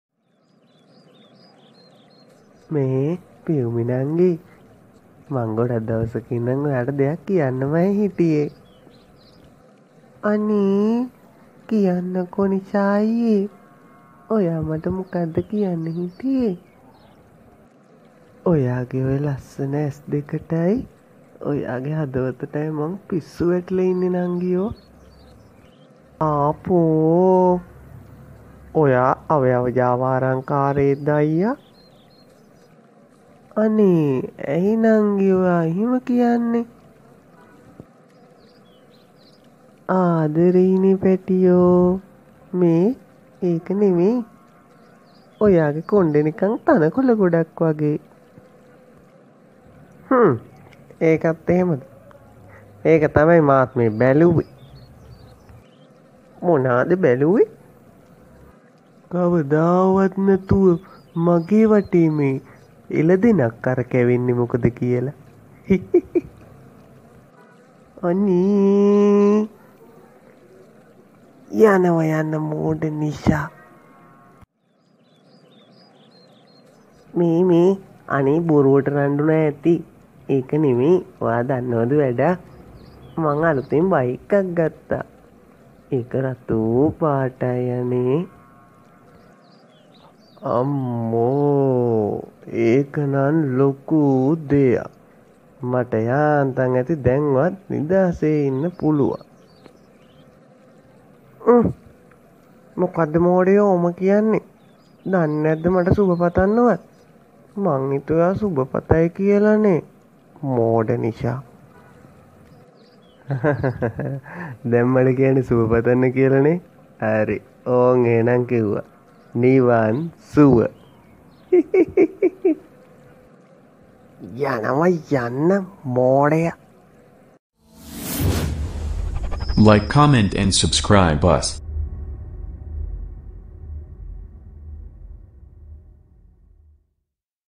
Greetings from Nisha | නිශාගේ සුභපැතිල්ල | Niwan suwa wadan | Best wish | Animation Tv | Sri Lanka
Hellow friends 🙂
If You Want To Enjoy Your Life
Plz ,

This video depicts a strange kind of greeting given to a young woman who buys a motorcycle.

යතුරුපැදියක් මිලට ගත තරුණියකට කරනු ලබන අමුතුම ආකාරයේ සුබපැතිල්ලක් ගැන මෙම වීඩියෝව මගින් නිරුපණය කෙරේ.
            බොහොම ස්තුතියි..!!